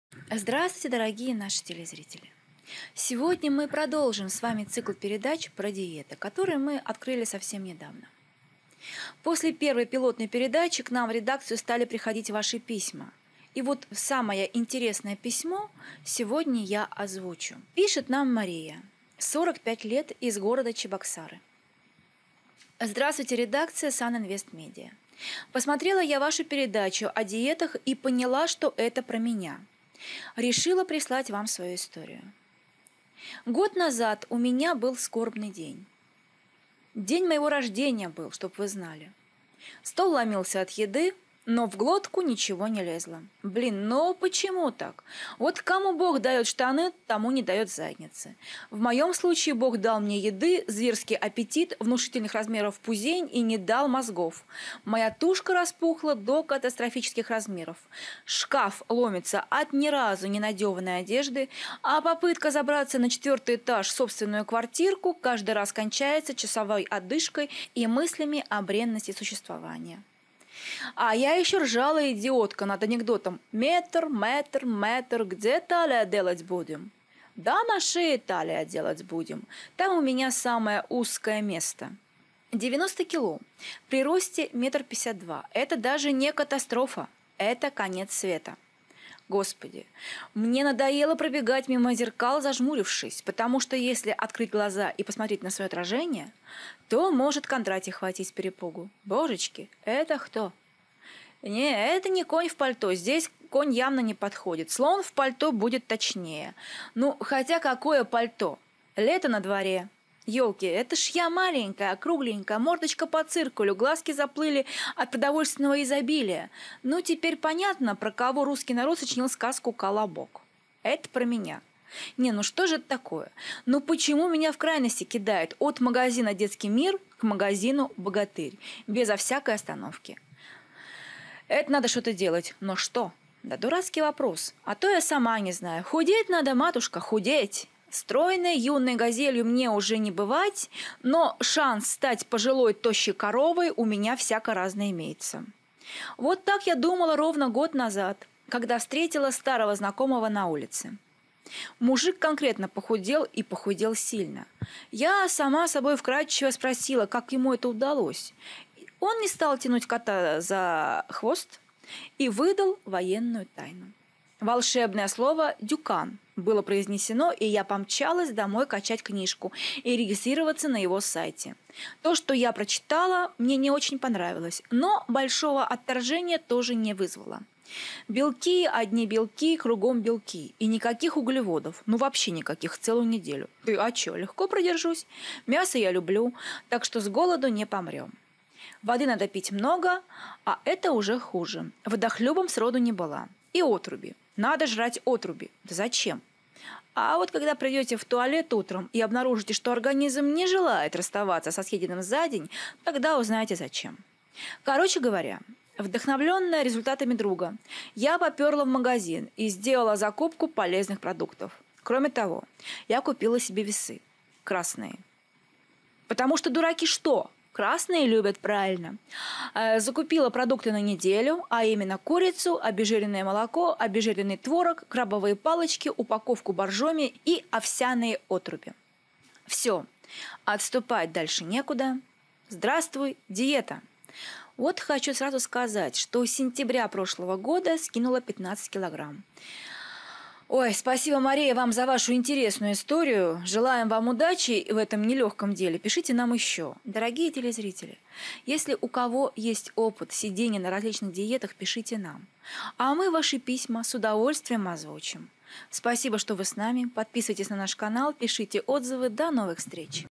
Диета или кто кого-2
Передовой опыт пышки ,похудевшей с 90 до 75 килограммов,почерпнутый из письма в редакцию.Если хотите.чтобы и ваш опыт был известен нашим зрителям,пишите нам.